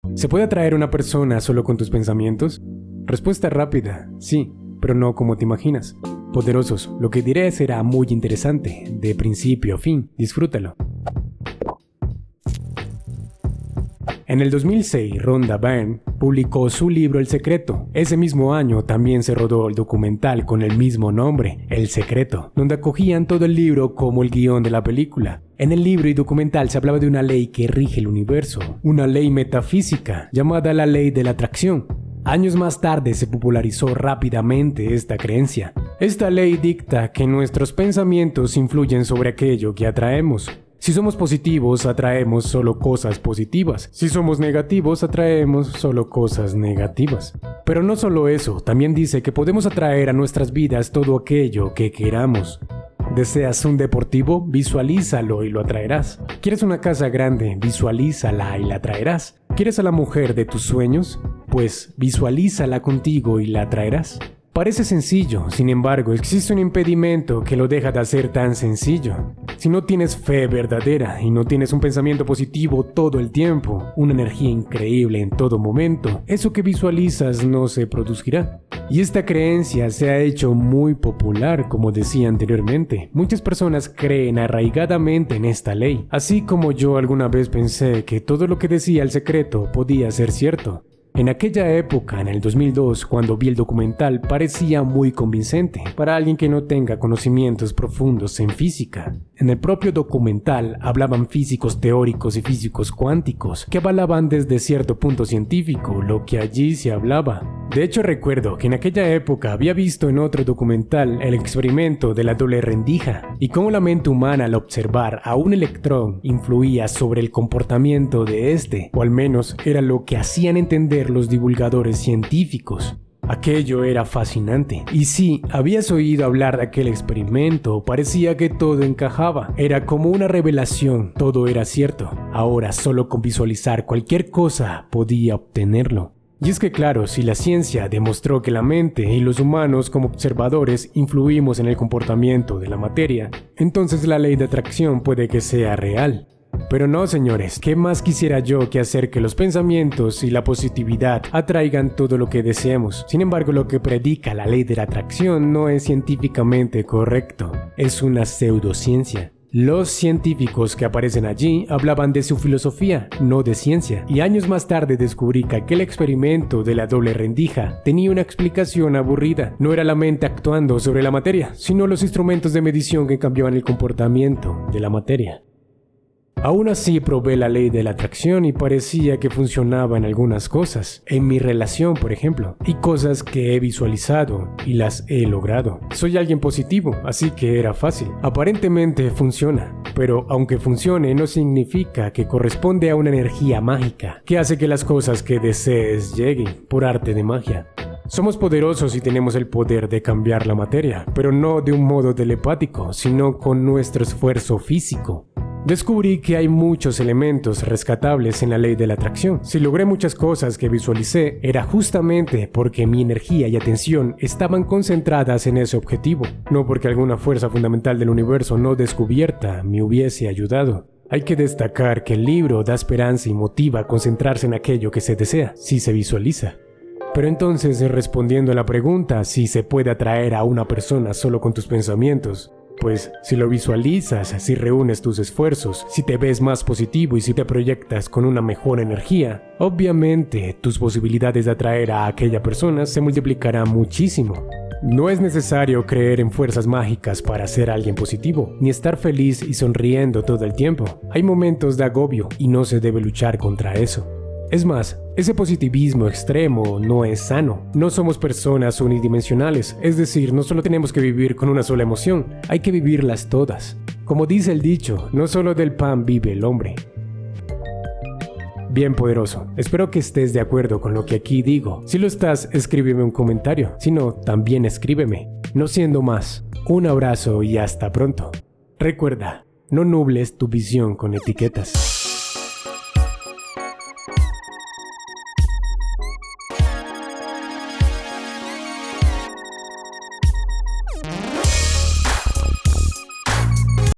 ¿Se Puede Atraer a Alguien con el Pensamiento? (La Verdad)
En este vídeo hablaré rápidamente sobre la Ley de la Atracción y lo que pienso de ella. ¿La ciencia demostró que la Ley de la Atracción existe?